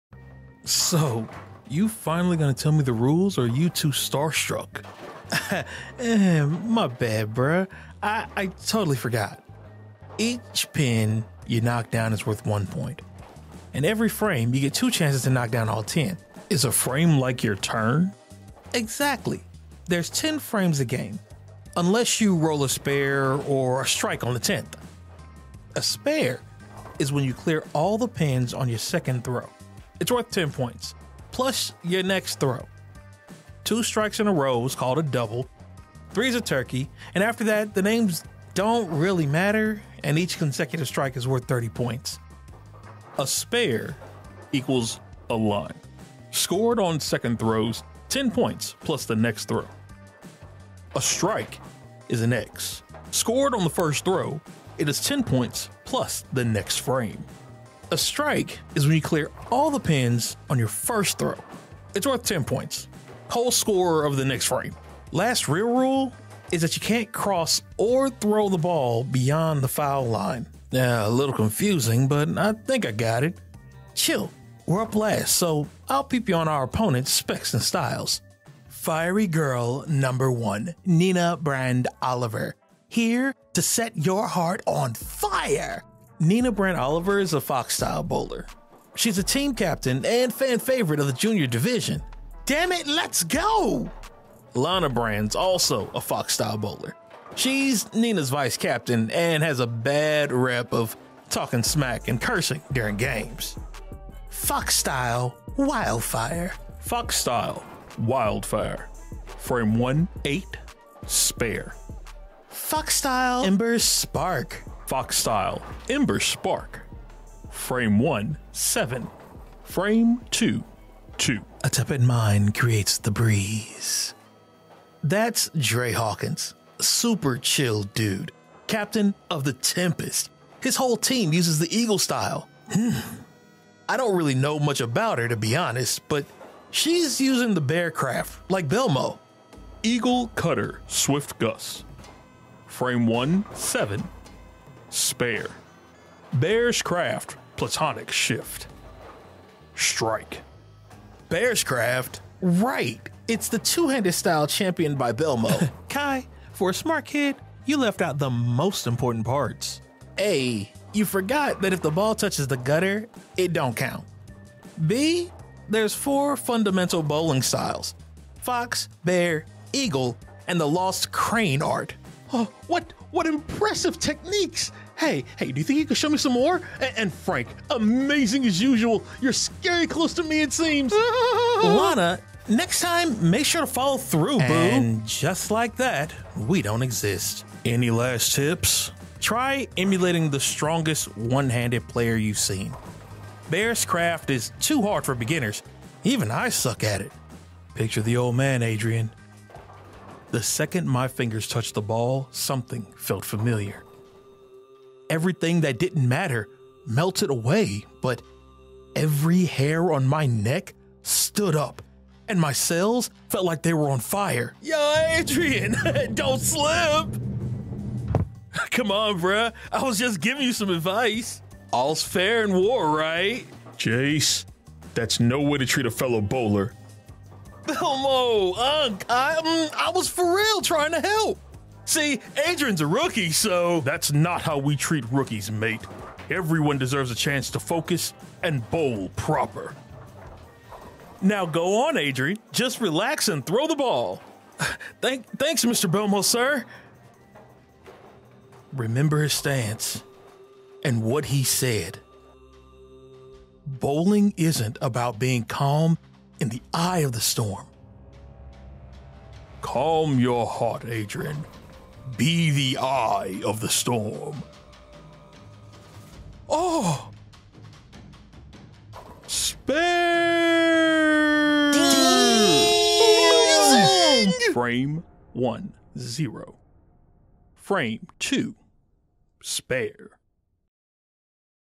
Help me Voice a Character in TENPINZ!
This is my bid to voice a character when TENPINZ becomes an anime! Please help by reaching out to the creators @Namiwear

Adrian Fisher, your average 15 year old kid discovers bowling, and a hidden technique that with the proper training can take him to the top.Through hyper focus, and calming his emotions, Adrian hones his crane technique against the evil forces of despair. All Merch and Physical copies can be found at Namiwear.com